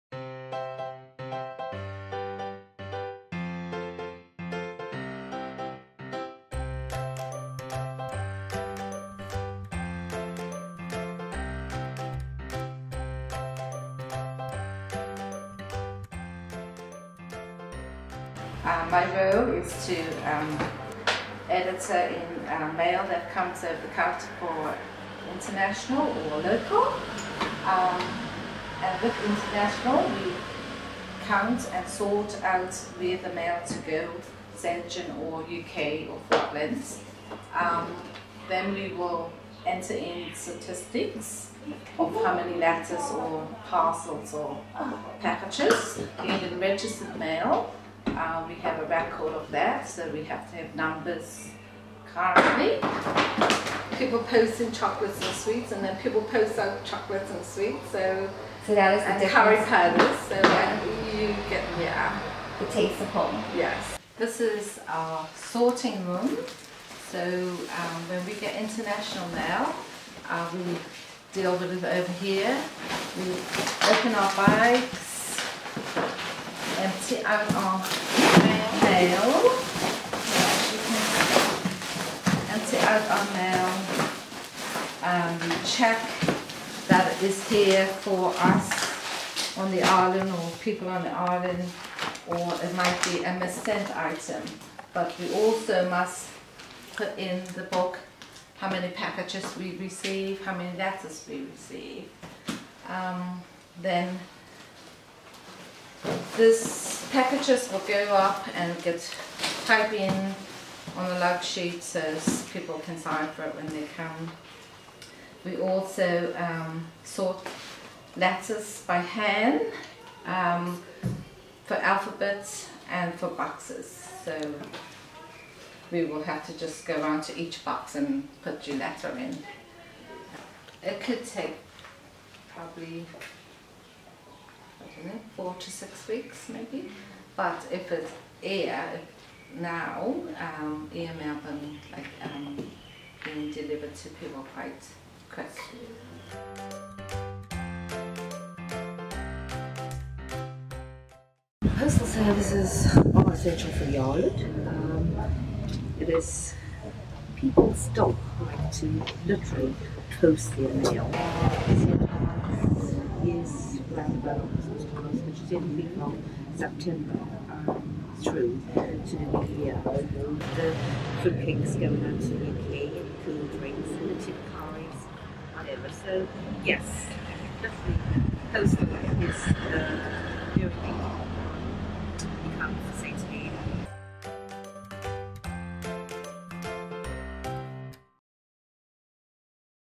St Helena's Post Office
Celebrating World Post Day
Taking a look at the postal service that supports the small and isolated St Helena Island community in the South Atlantic Ocean.